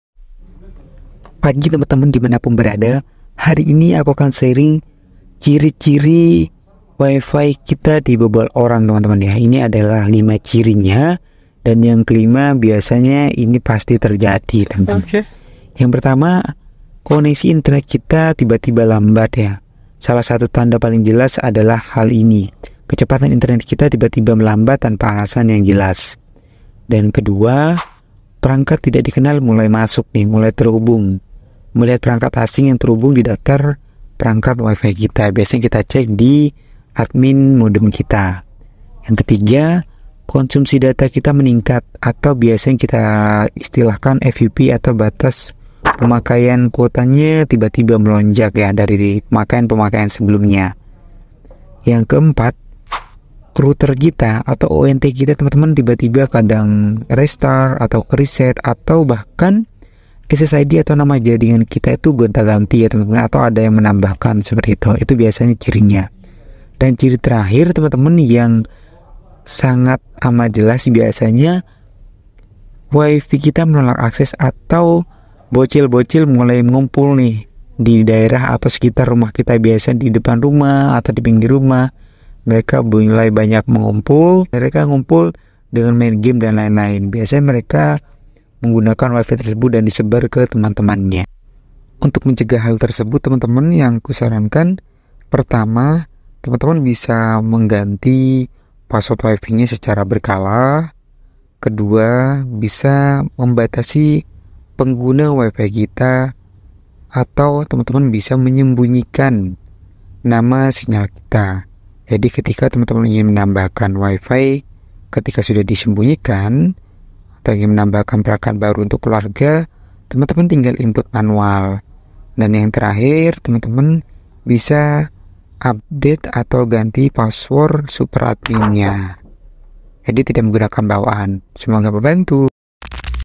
5 Tanda wifi kita dibobol orang
ini adalah 5 ciri wifi kita dipakai orang, menjadi lambat, dll.
Ohya untuk teman teman yang mau tanya tanya seputar Telkomsel atau mau pasang baru WiFi indihome bisa klik link dibawah ya 👇

Created by l AkuEdy 2024